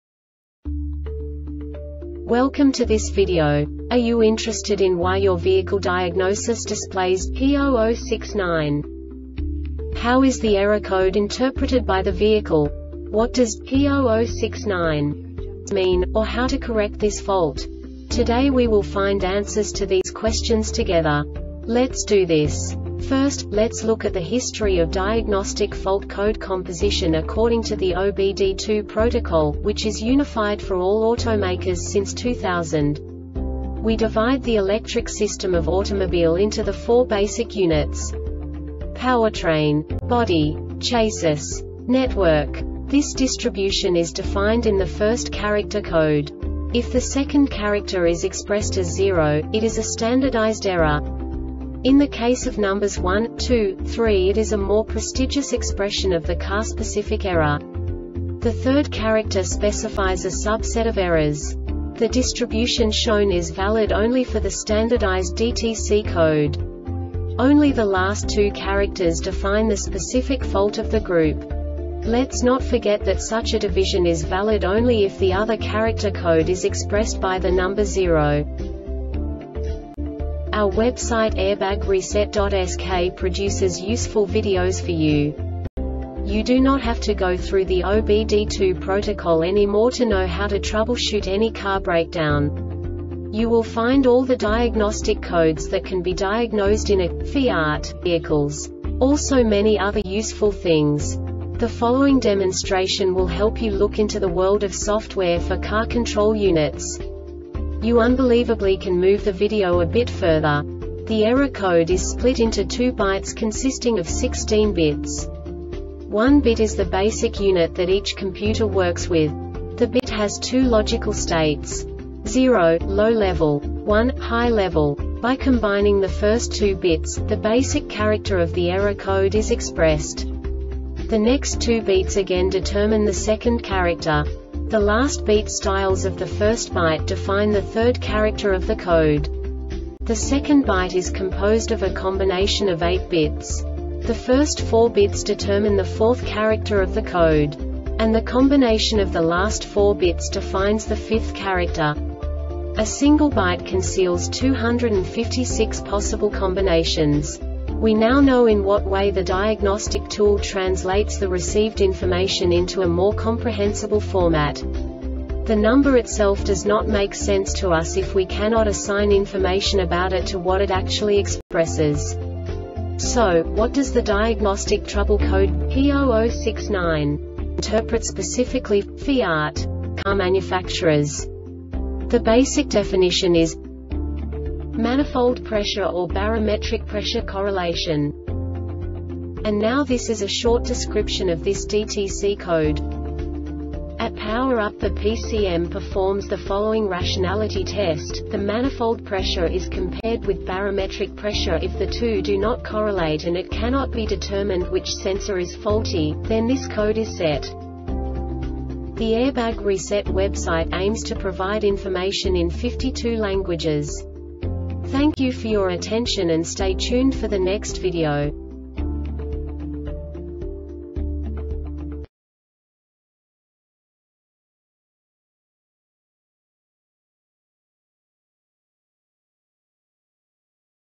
DTC Fiat P0069 Short Explanation
Contents:
0:21 Basic DTC analysis according to OBD2 protocol standard.
1:48 Insight into programming
2:58 A diagnostically understandable description of the Diagnostic Trouble Code
3:05 Basic error definition

Make:
FIAT
Code:
P0069
Definition:
Manifold Pressure or Barometric Pressure Correlation
Description:
At power-up the PCM performs the following rationality test, the manifold pressure is compared with barometric pressure. If the two do not correlate and it cannot be determined which sensor is faulty, then this code is set.